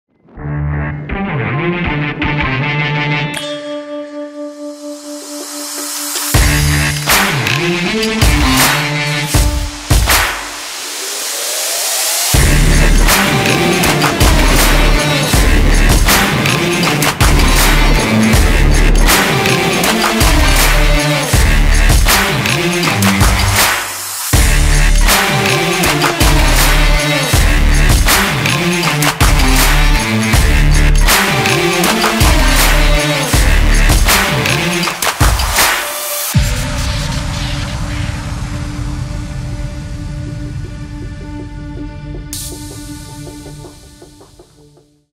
装備品紹介 護衛艦あたご
【平和をまもる】
イージス・システムを装備した護衛艦「あたご」
日本の領域をまもる、ミサイル防衛の要です！